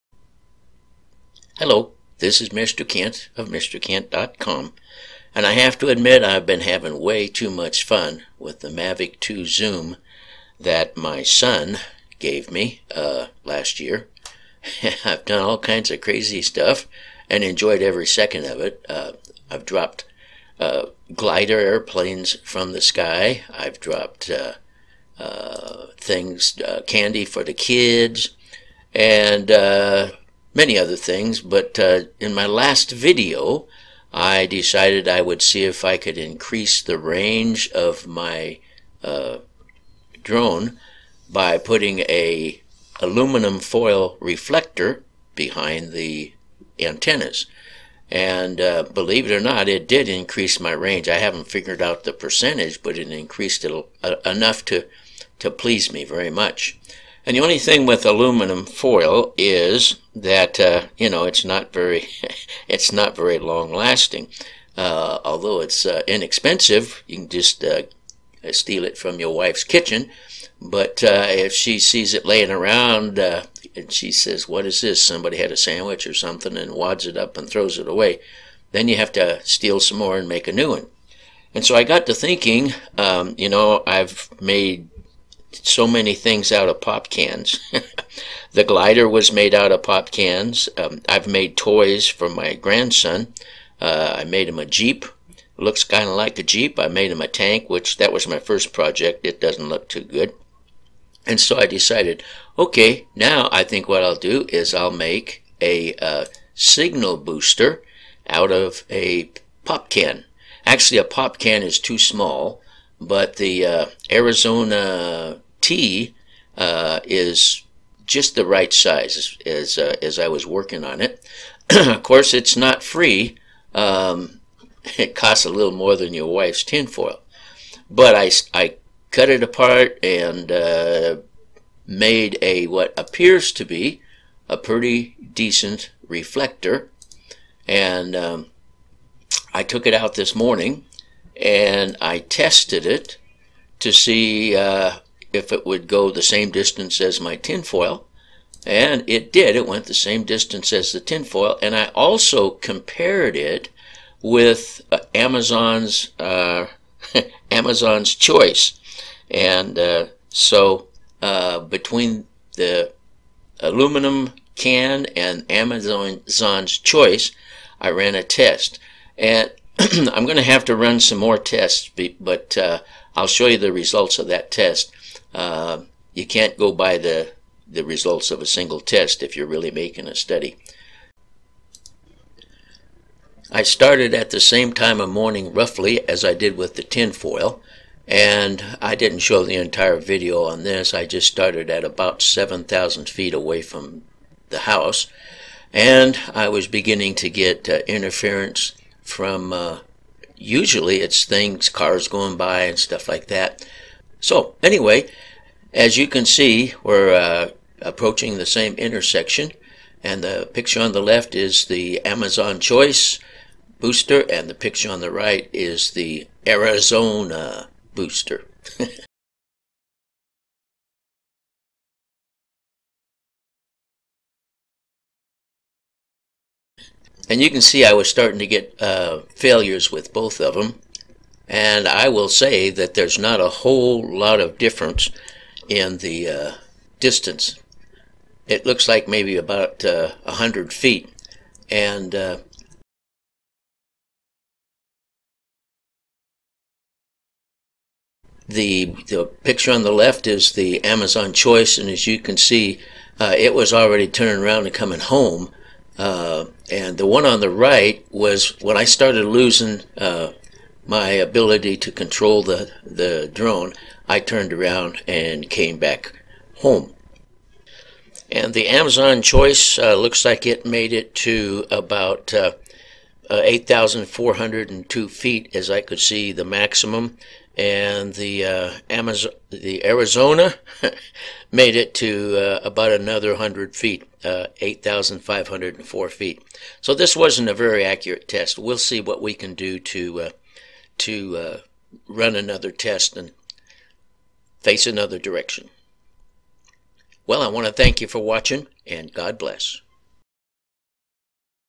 MAVIC POP CAN SIGNAL BOOSTER
In my last video I used some aluminum foil as a signal booster and it seemed to work fine. In this video I switch to using a pop can as a signal booster. And, I compare my pop can with the standard $10 booster sold by Amazon.